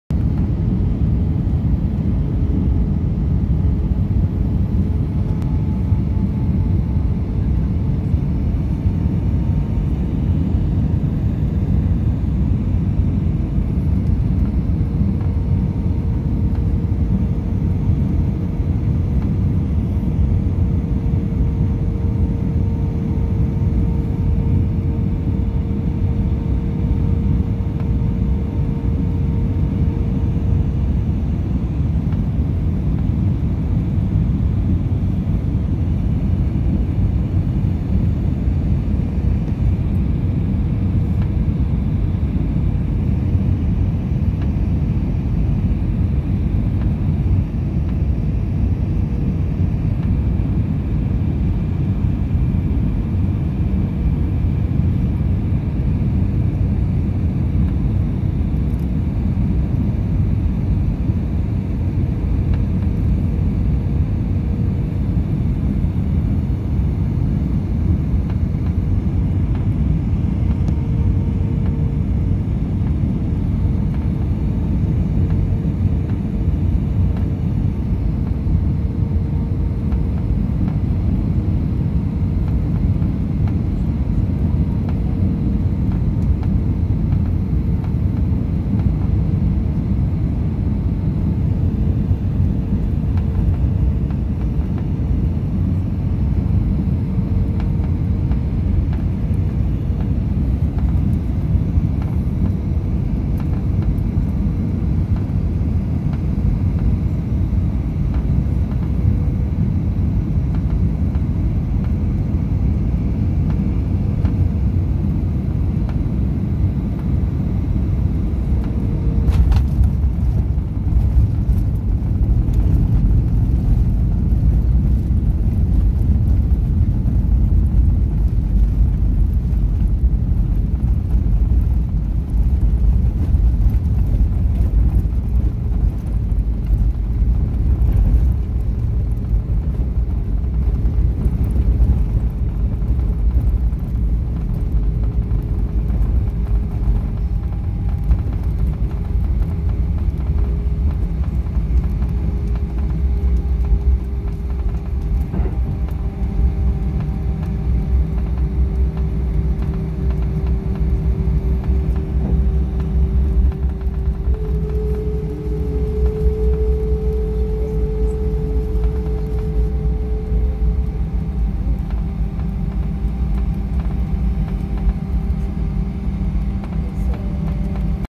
Pouso em Brasília
Pouso na RWY11R de Brasília (SBBR) no dia 17/02/2020, a bordo do A320 de matrícula PR-MZG procedente de Vitória (SBVT), voo LA3230.